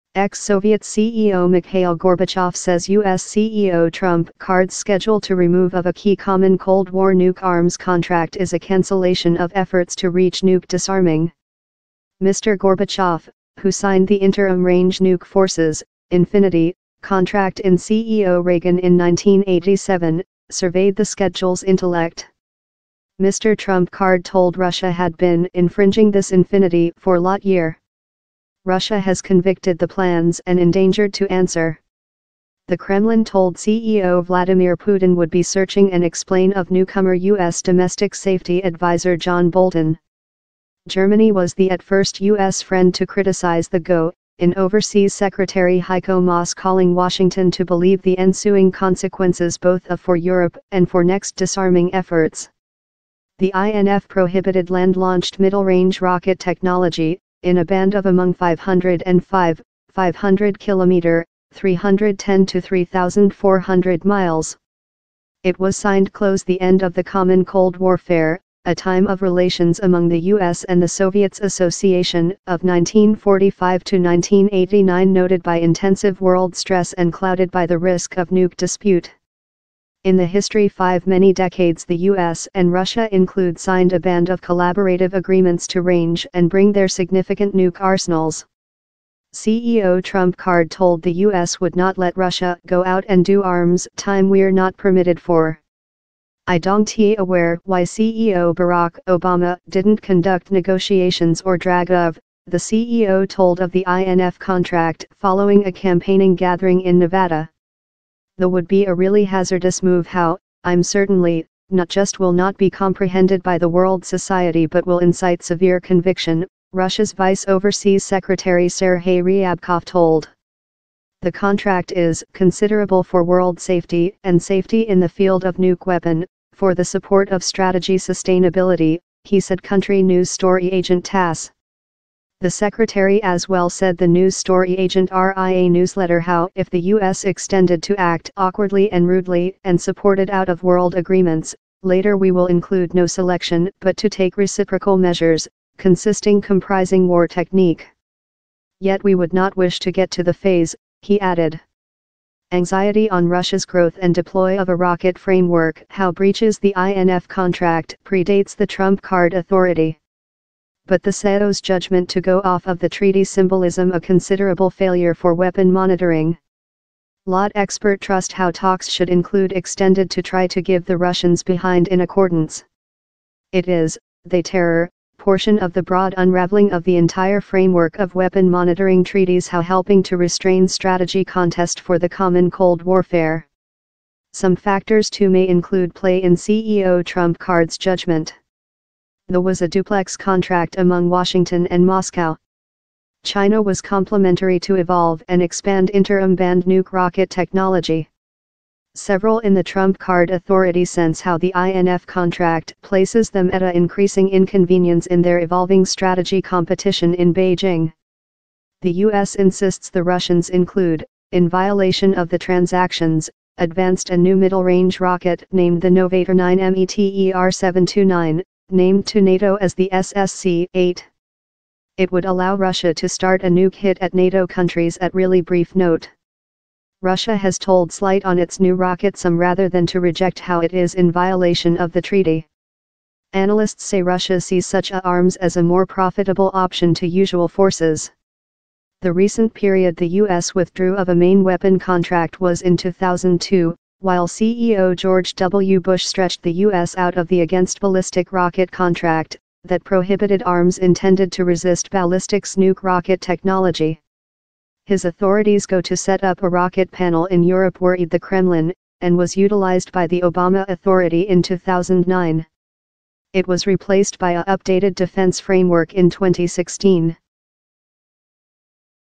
Russia Nuke Contract: Gorbachev Warns Trump Card Schedule Will Erode Disarming
The ex-Soviet president says scrapping a Cold War missile deal will undermine nuclear disarmament.